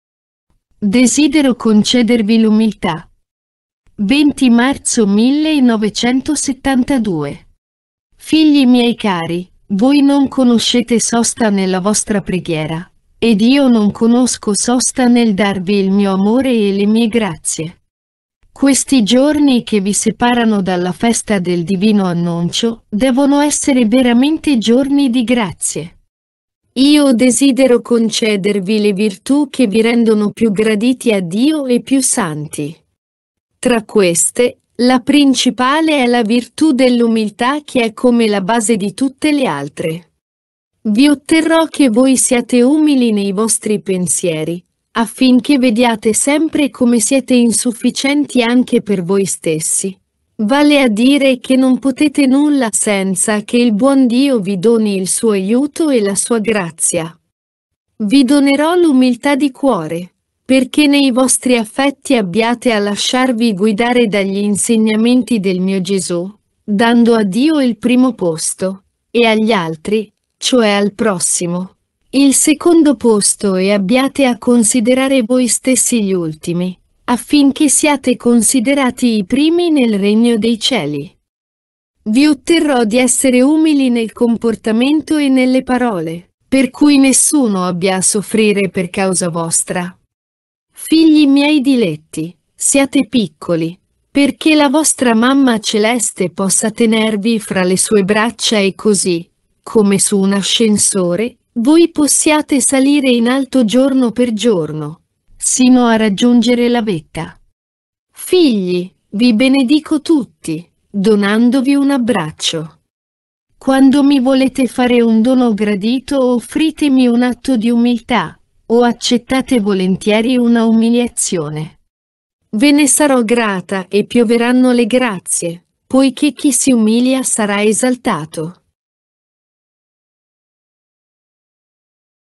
Desidero concedervi l'umiltà
MARIA MADRE E MAESTRA 5. Mamma Carmela 20 marzo 1972.

Figli miei cari, voi non conoscete sosta nella vostra preghiera ed io non conosco sosta nel darvi il mio amore e le mie grazie. Questi giorni che vi separano dalla festa del divino Annuncio, devono essere veramente giorni di grazie. Io desidero concedervi le virtù che vi rendono più graditi a Dio e più santi. Tra queste, la principale è la virtù dell'umiltà che è come la base di tutte le altre.
Vi otterrò che voi siate umili nei vostri pensieri, affinché vediate sempre come siete insufficienti anche per voi stessi, vale a dire che non potete nulla senza che il buon Dio vi doni il suo aiuto e la sua grazia.
Vi donerò l'umiltà di cuore, perché nei vostri affetti abbiate a lasciarvi guidare dagli insegnamenti del mio Gesù dando a Dio il primo posto, e agli altri, cioè al prossimo, il secondo posto e abbiate a considerare voi stessi gli ultimi affinché siate considerati i primi nel regno dei cieli.
Vi otterrò di essere umili nel comportamento e nelle parole, per cui nessuno abbia a soffrire per causa vostra.
Figli miei diletti, siate piccoli, perché la vostra Mamma celeste possa tenervi fra le sue braccia e così, come su un ascensore, voi possiate salire in alto giorno per giorno, sino a raggiungere la vetta.
Figli, vi benedico tutti, donandovi un abbraccio.
Quando mi volete fare un dono gradito offritemi un atto di umiltà o accettate volentieri una umiliazione. Ve ne sarò grata e pioveranno le grazie, poiché chi si umilia sarà esaltato.